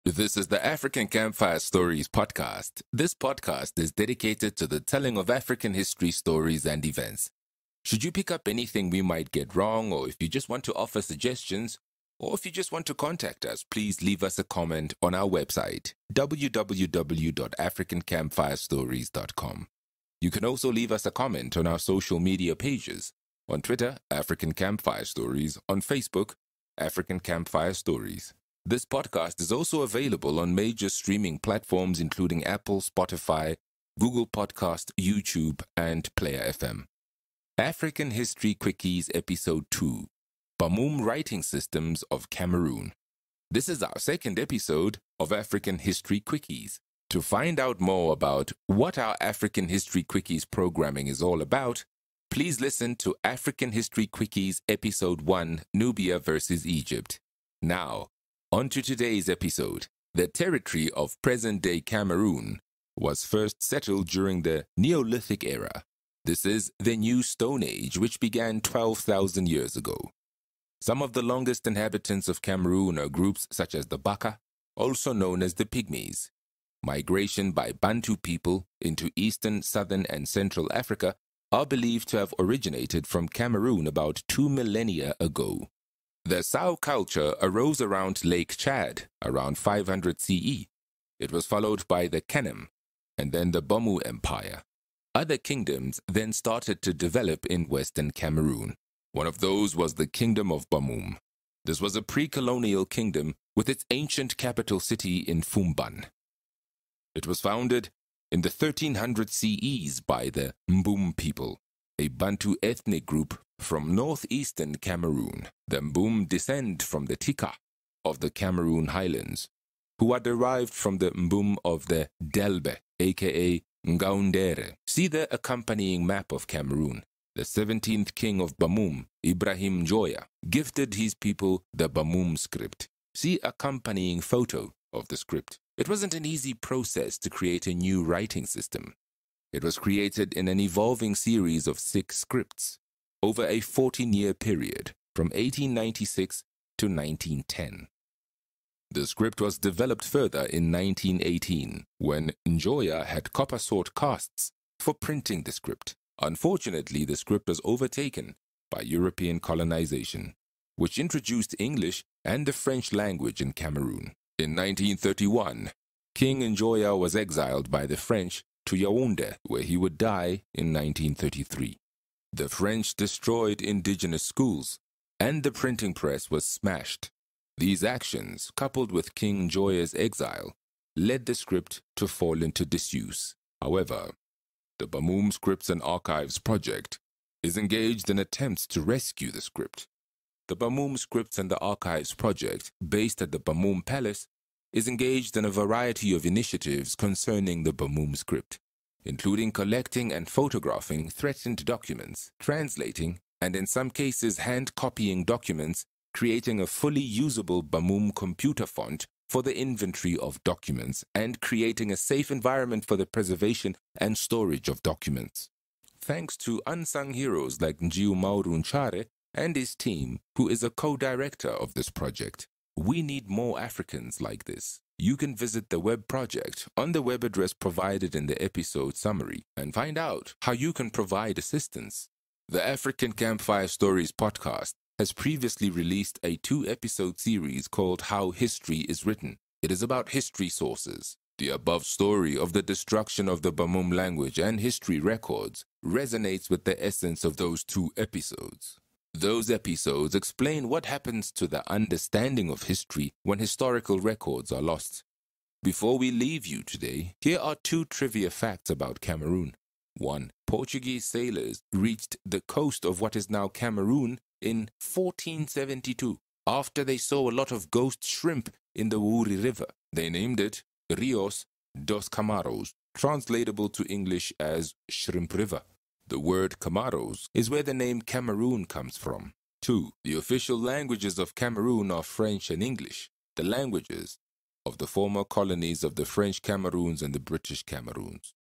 African History Quickies – Episode 2 – The Bamum Writing System of Cameroon
On this quickie episode we discuss the Bamum script of Cameroon: how was it created; how was it lost; AND what is being done to restore it.

Featuring: Cameroon; Bamum script; the Tikar; the Mbum; the British; the French; King Njoya; and much more

Facebook: African Camp Fire Stories